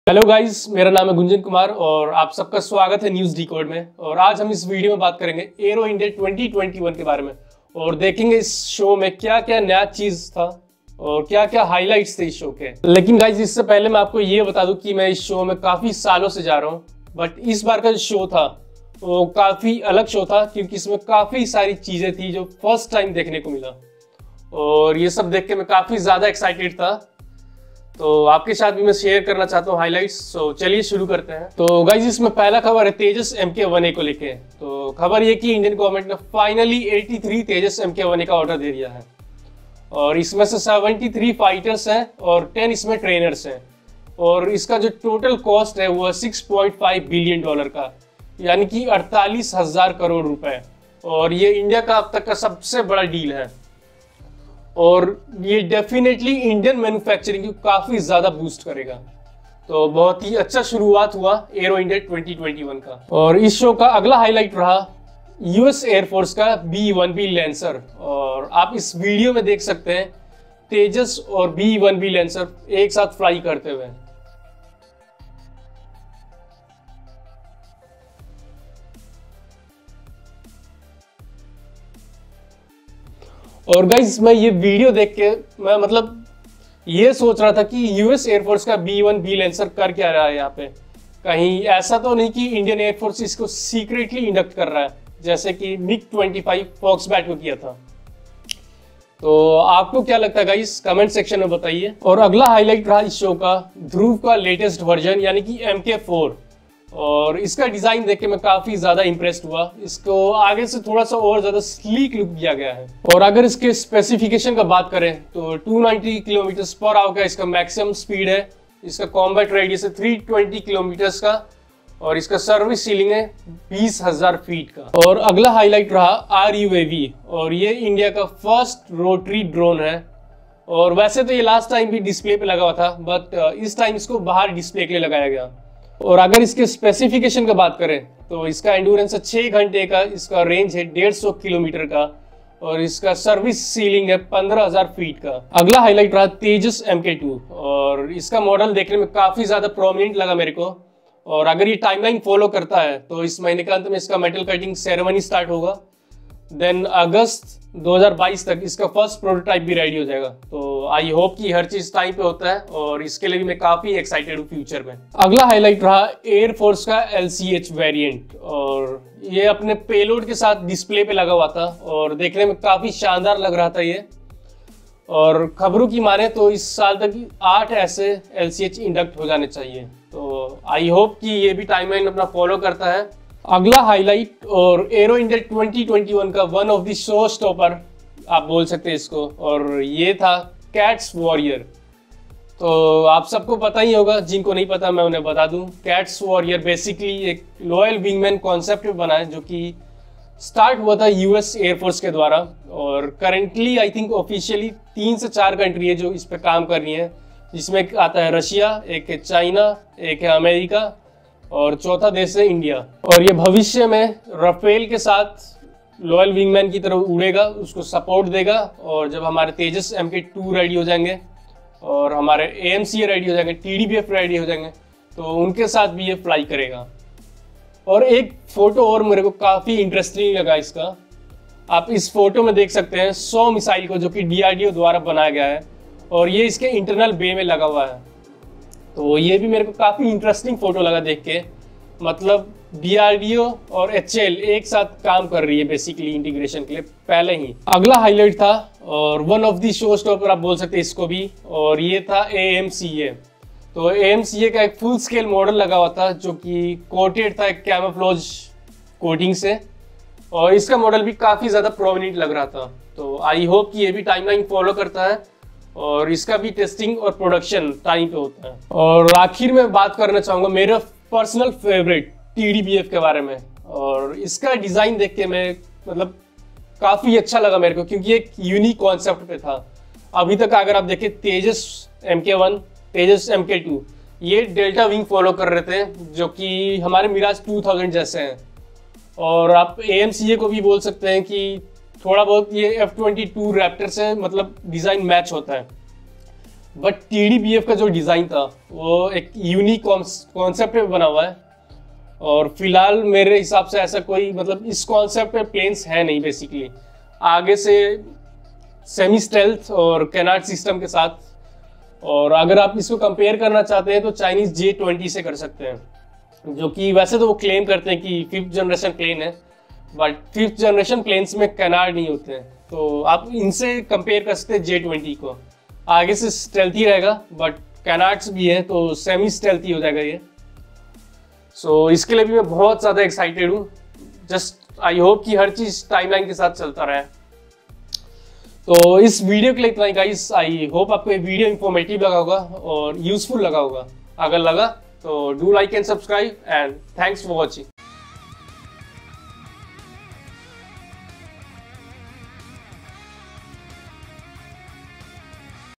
AERO INDIA 2021 Highlight | Indian Defence news latest
Time Stamp :-
0:00 - AERO INDIA 2021 introduction
00:32 - LCA Tejas latest news
01:05 - US B-1B bomber in AERO India
01:43 - Dhruv helicopter mark 4
02:06 - RUAV 200 HAL
02:26 - Tejas MK 2 latest news
02:47 - LCH latest news
03:10 - Cats Warrior HAL latest news
04:12 - AMCA fighter jet latest news
04:32 - TEDBF latest news